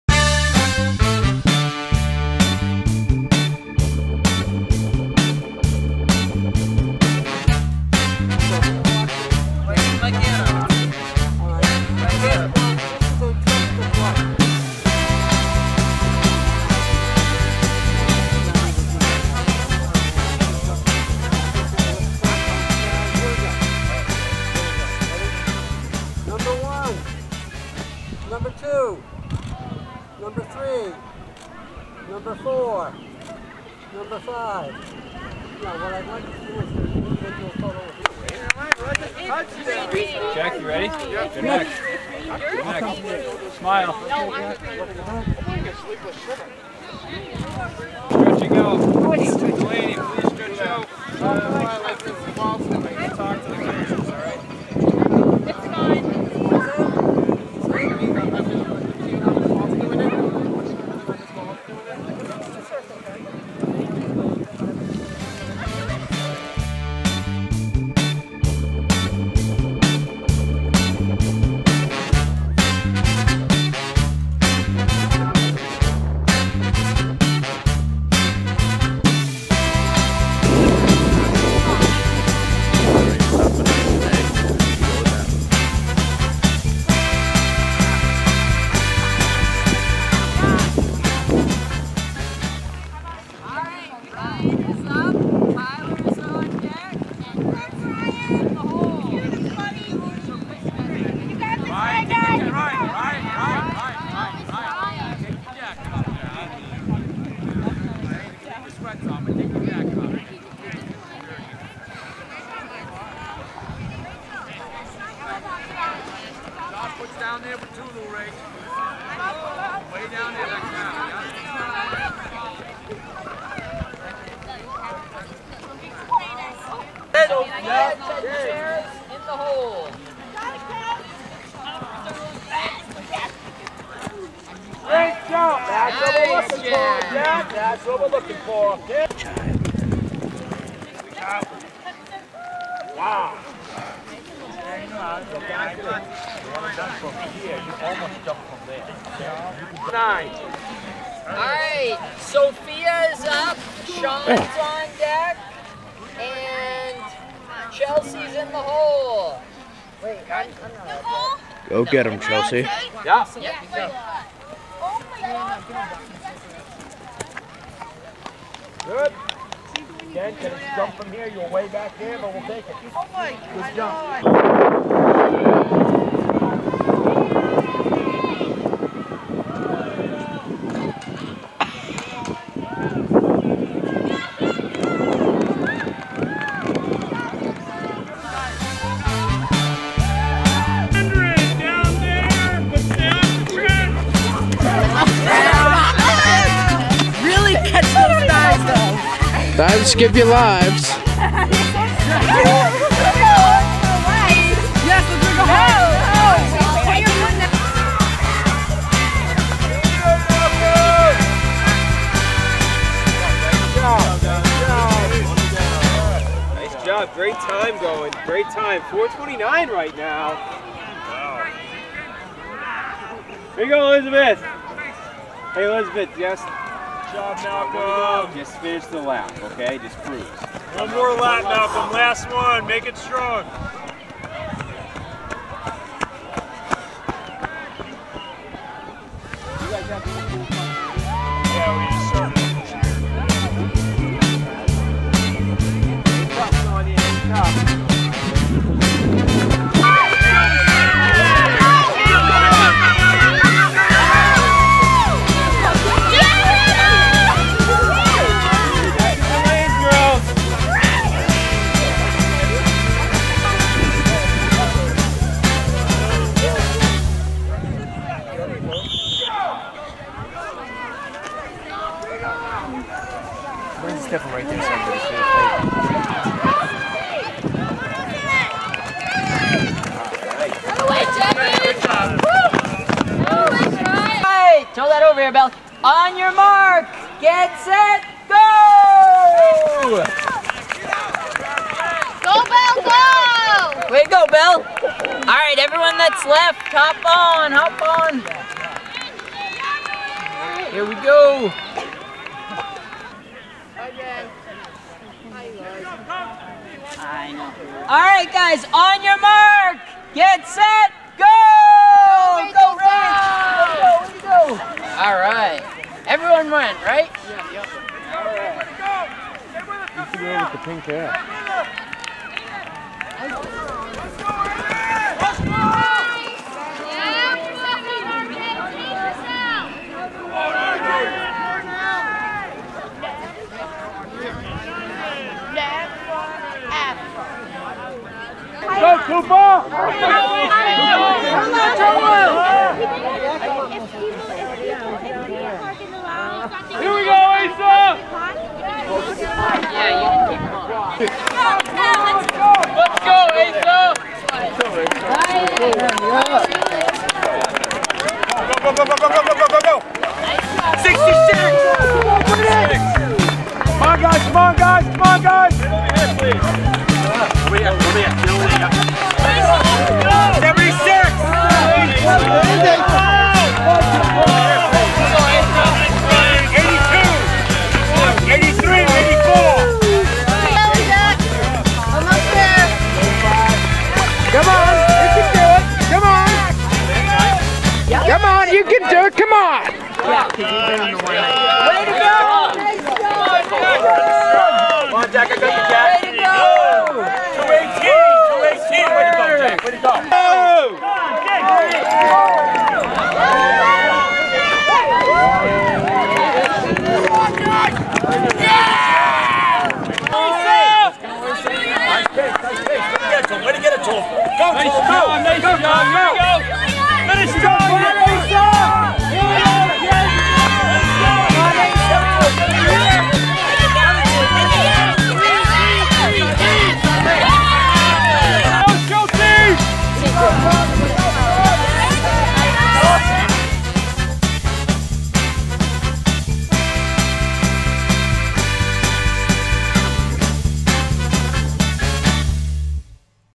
Nauset Unified Track - a few clips from the May 2 home meet.
Camera: Ben Morgan, Thomas Schultze